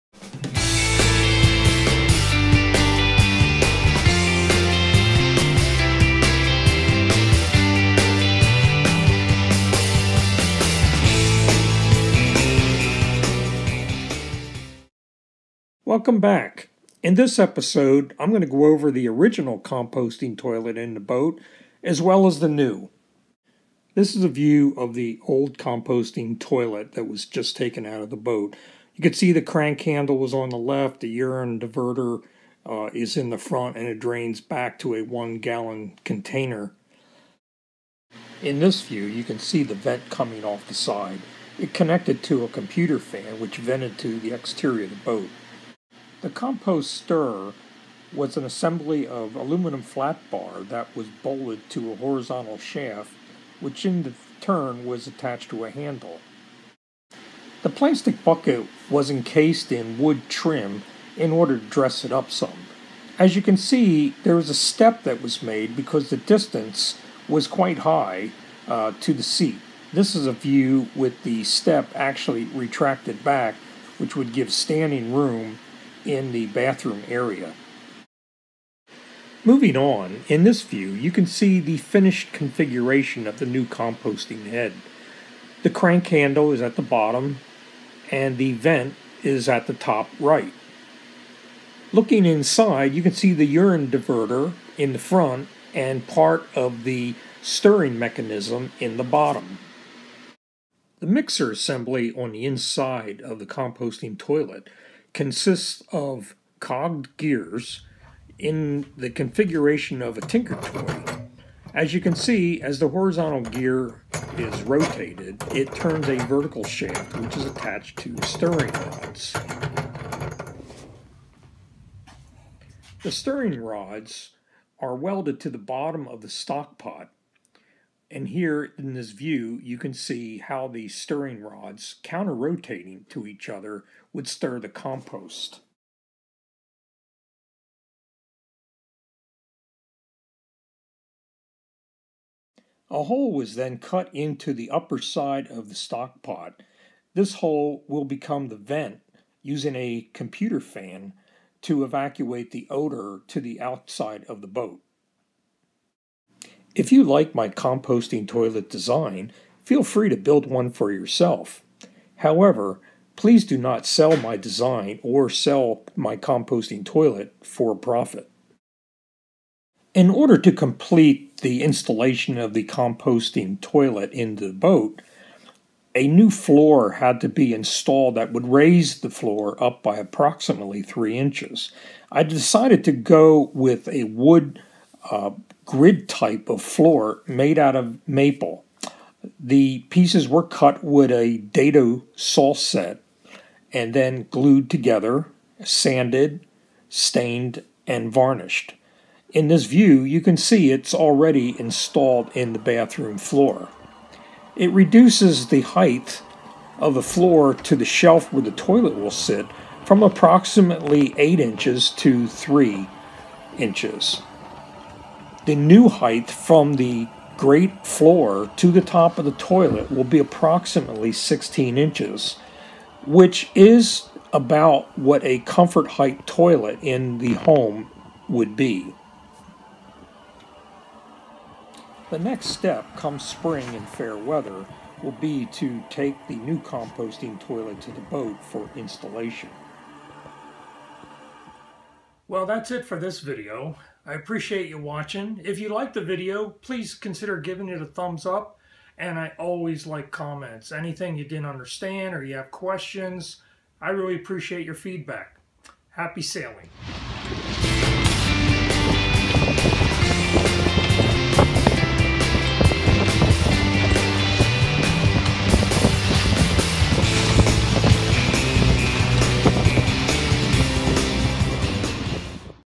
Sailing Vessel Southern Lady - Building a Composting Head (1-7-16 part 8)
Here are details of my new composting toilet.  Last year I build and installed a composting head for aboard my boat.  While the commercial units were quite impressive, their overall height combined with the floor height would be too great.  I elected to build a prototype to see if I liked the concept.  I was impressed with the low maintenance of a composting head.  No more odors, no pomp-outs, and no clogs.

Based on the success of the prototype, I decided to build a more permanent unit.  I researched the "stirring" designs but could not find one I liked. As a result I designed and built my own.  It solves the height problem.  The mixing mechanism is low profile which lowers the seat height to a "comfort" toilet level.

Feel free to use my design to build your own.  However, please do not use my design for profit.